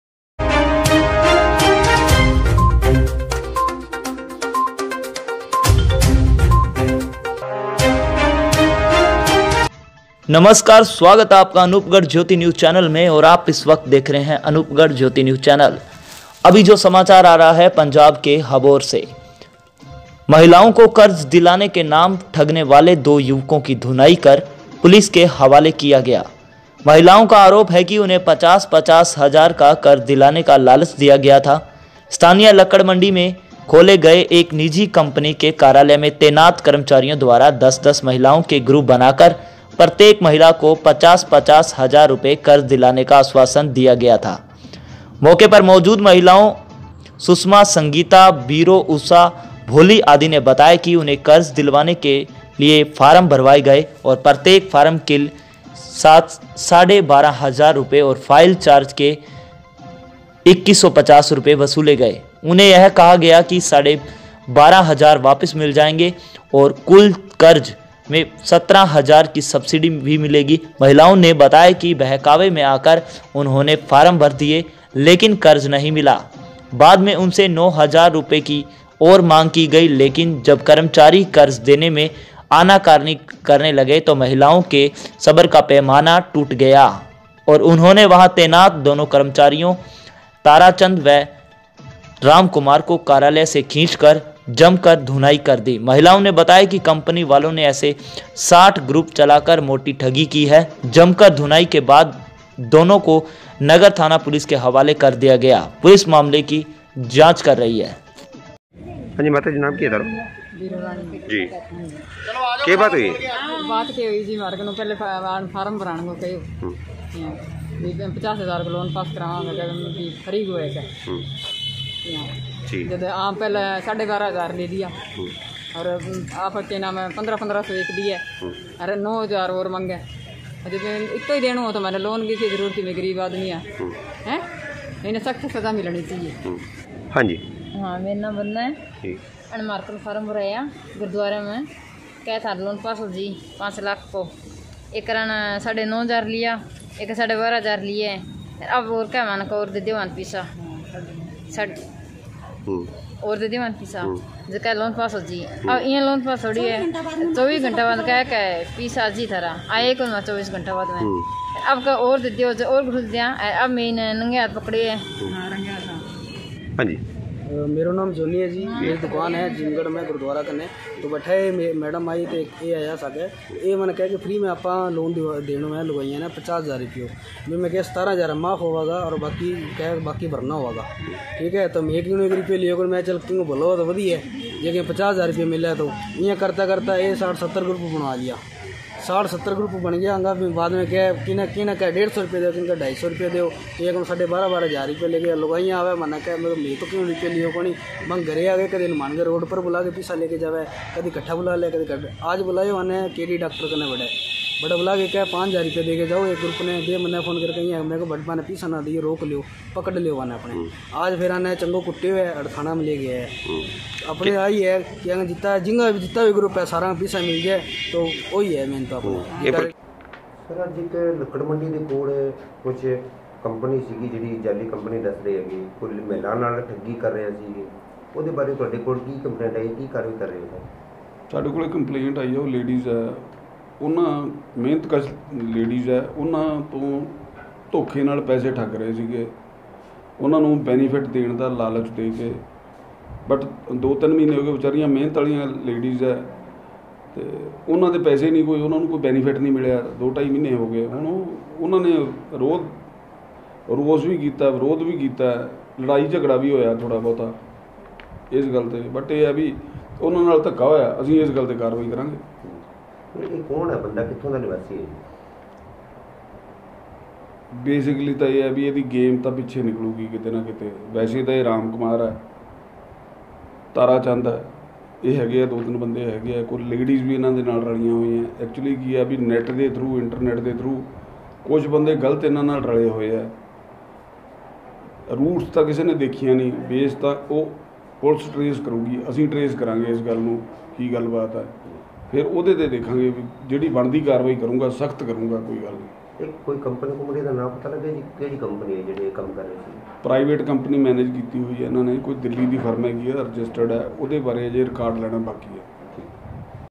अबोहर मे महिलाओं को कर्ज दिलाने के नाम ठगने वाले दो युवकों की धुलाई
महिलाओं को कर्ज दिलाने के नाम ठगने वाले दो युवकों की धुनाई कर पुलिस के हवाले किया गया है। महिलाओं का आरोप है कि उन्हें पचास पचास हजार का कर्ज दिलाने का लालच दिया गया था।
 स्थानीय लक्कड़ मंडी में खोले गए एक निजी कंपनी के कार्यालय में तैनात कर्मचारियों द्वारा दस-दस महिलाओं के ग्रुप बनाकर प्रत्येक महिला को पचास-पचास हजार रुपये कर्ज दिलाने का आश्चसन दिया था। मौके पर मौजूद महिलाओं सुषमा, संगीता, बीरो, उषा, भोली आदि ने बताया कि उन्हें कर्ज दिलवाने के लिए फार्म भरवाए गए और प्रत्येक फार्म के साथ साढ़े 12 हजार रुपये और फाइल चार्ज के हरेक से 2150 रुपये वसूले गए। उन्हें यह कहा गया कि साढे 12 हजार वापिस मिल जाएंगे और कुल कर्ज में 17 हजार की सब्सिडी भी मिलेगी। महिलाओं ने बताया कि बहकावे में आकर उन्होंने फार्म भर दिए लेकिन कर्ज नहीं मिला। बाद में उनसे 9 हजार रुपये की और मांग की गई। लेकिन जब कर्मचारी कर्ज देने में आनकानी करने लगे तो महिलाओं के सब्र का पैमाना टूट गया और उन्होंने वहां तैनात दोनो कर्मचारियों ताराचंद व रामकुमार को कार्यालय से खींच कर जम कर धुनाई कर दी। महिलाओं ने बताया कि कंपनी वालों ने ऐसे 60 ग्रुप चलाकर मोटी ठगी की है। जमकर धुनाई के बाद दोनों को नगर थाना पुलिस के हवाले कर दिया गया। पुलिस मामले की जांच कर रही है।